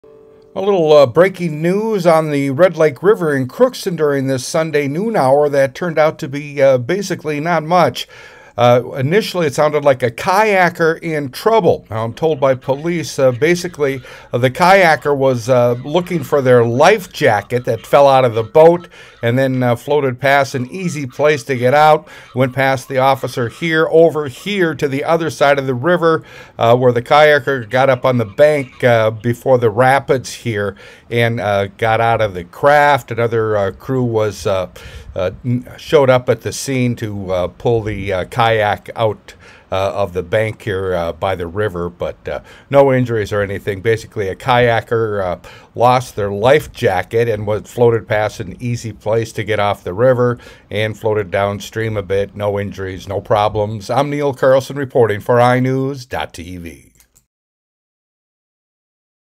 Kayak Incident On Red Lake River In Crookston, Minnesota
CROOKSTON, MN (iNewZ.TV) We had a small incident involving a kayaker on the Red Lake River at Crookston during this Sunday noon hour.